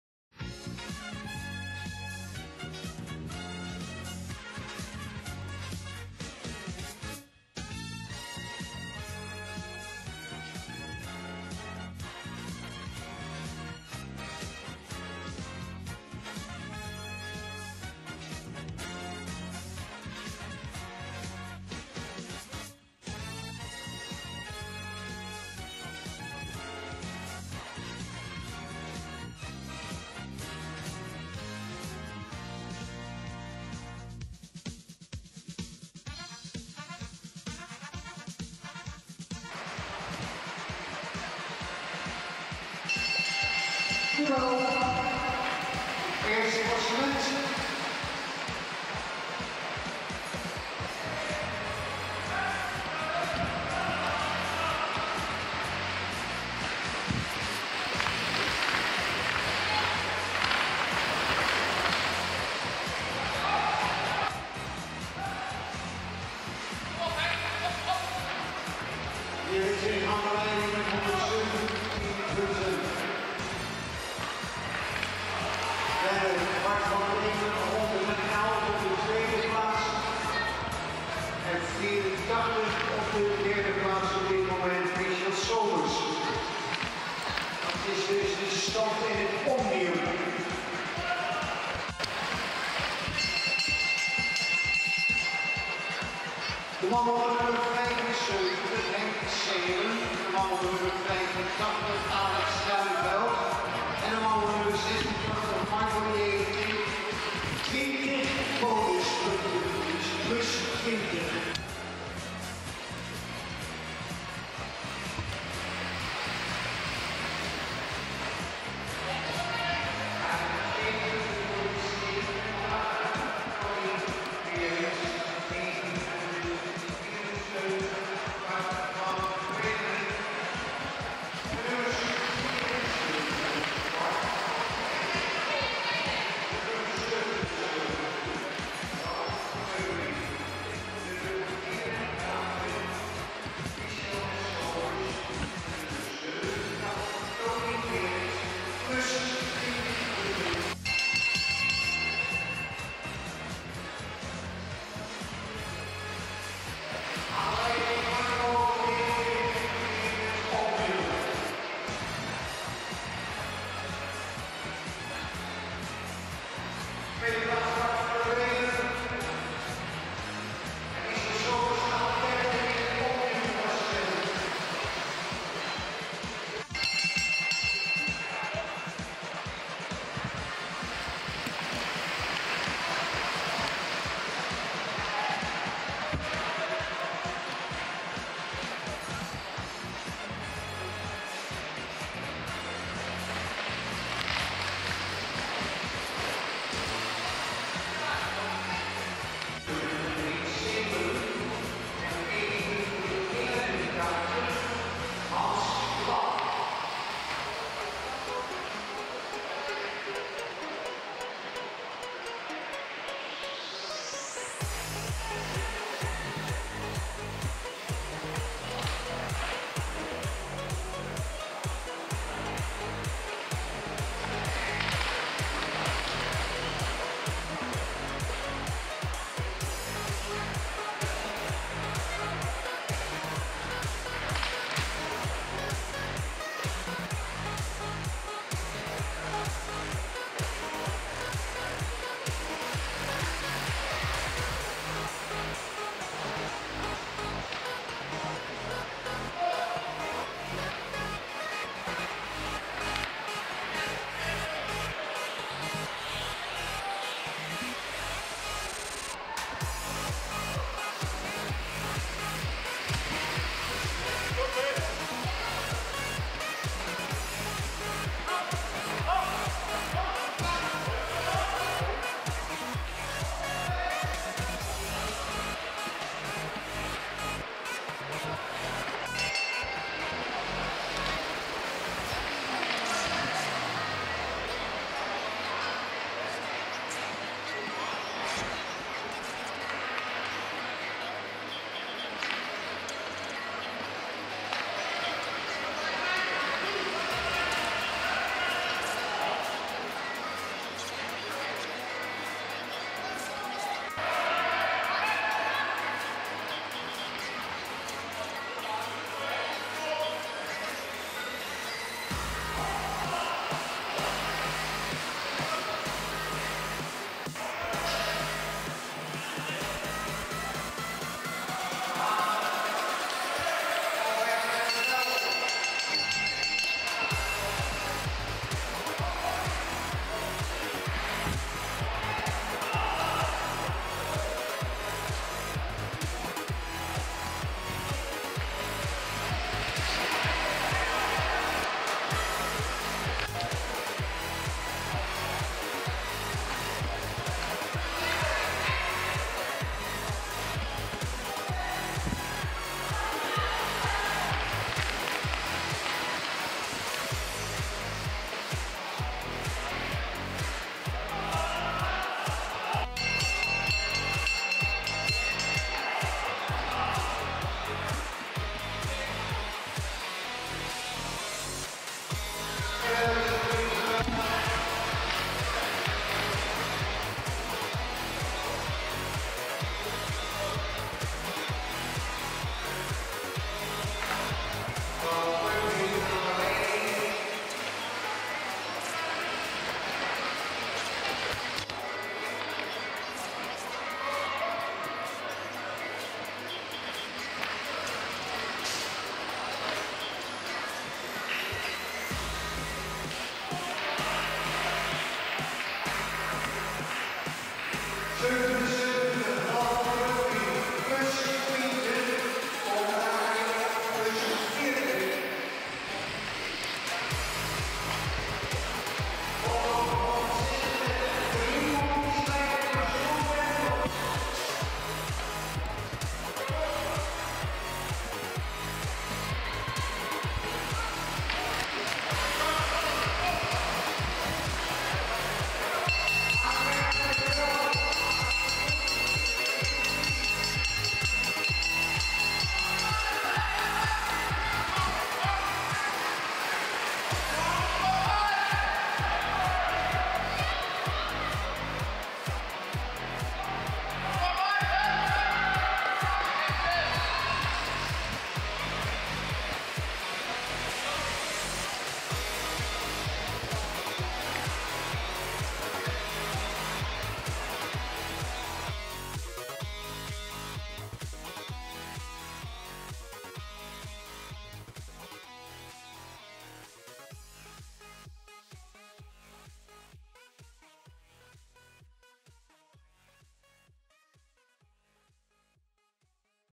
20180107 NK Baan Puntenkoers
Uitgebreid verslag van de Puntenkoers tijdens het NK Baan voor Masters 50+ op 7 januari 2018 in het Velodrome te Amsterdam.
Een spannende wedstrijd met sterke mannen.